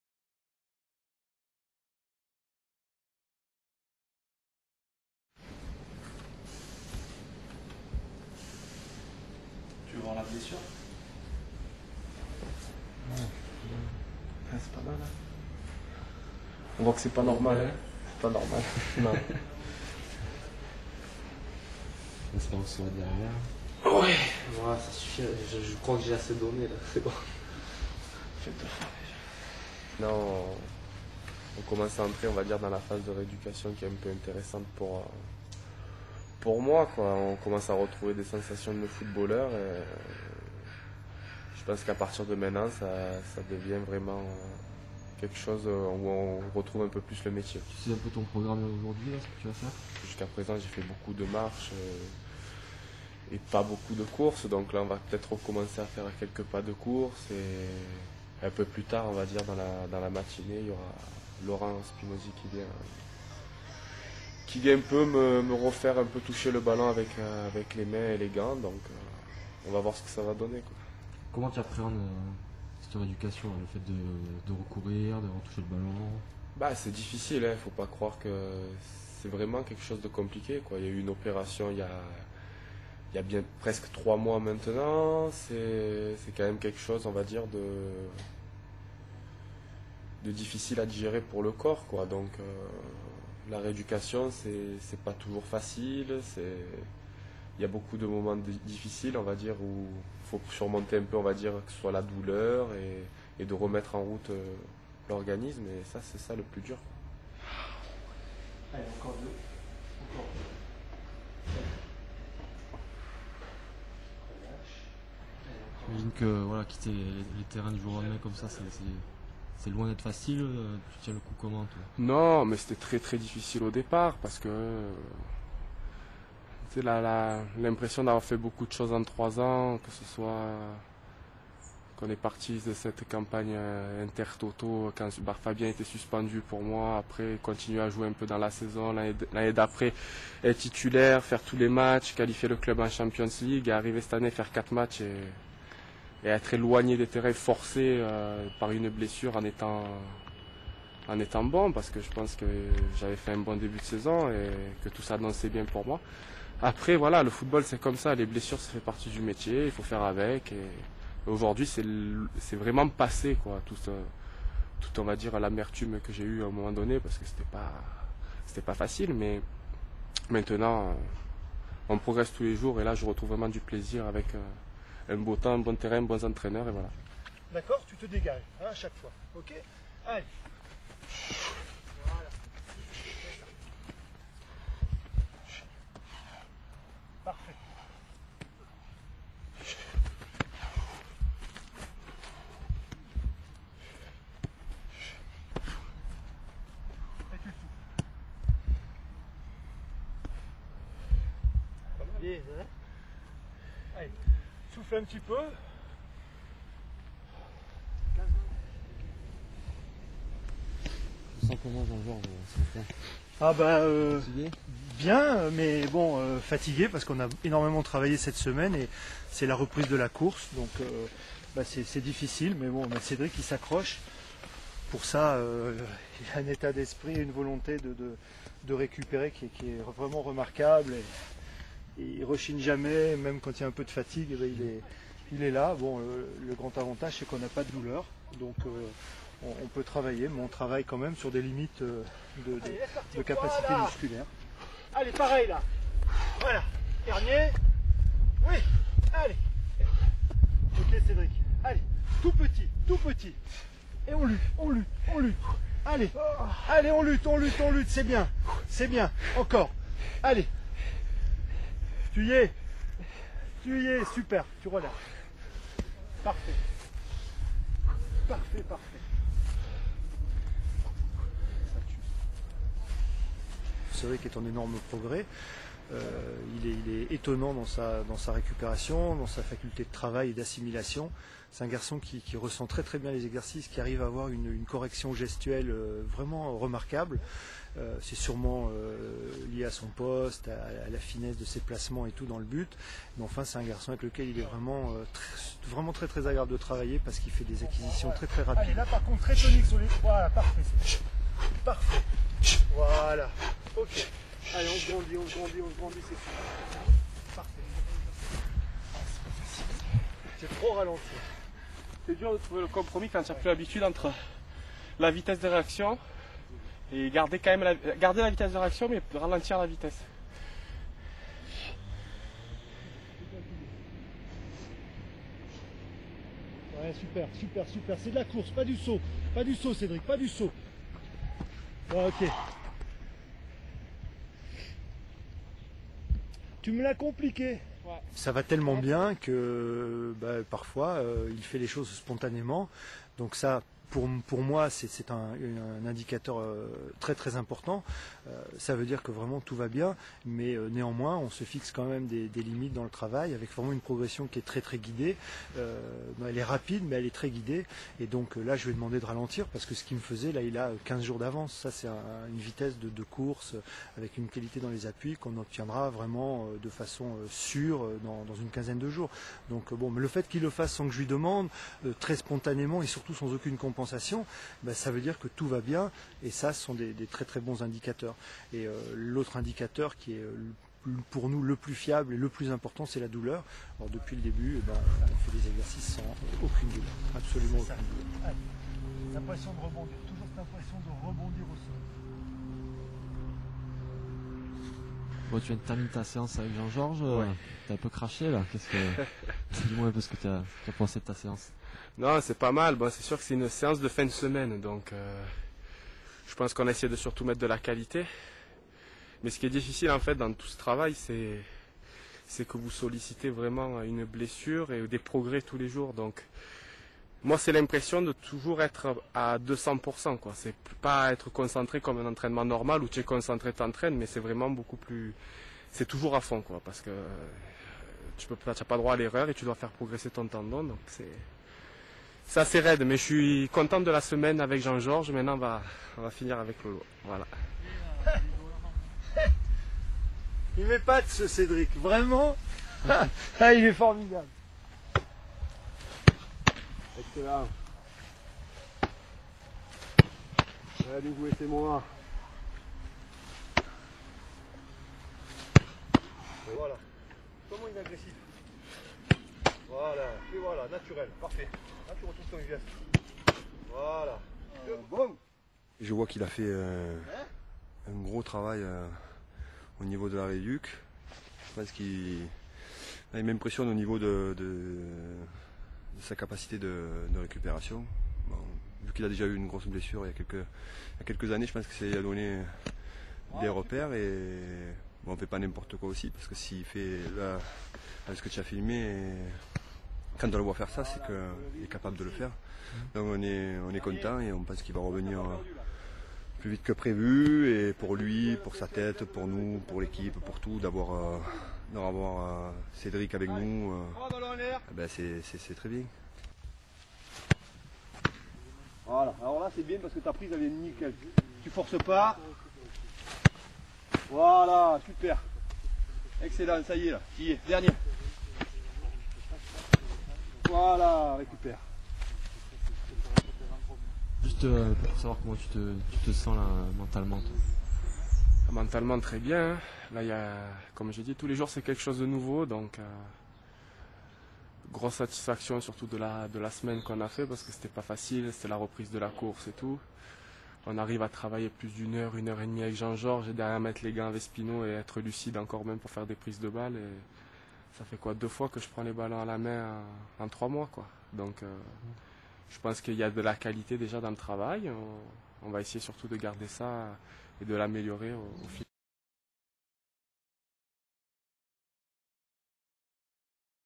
OM CARASSO REEDUCATION
Video de carasso pendant sa remise en forme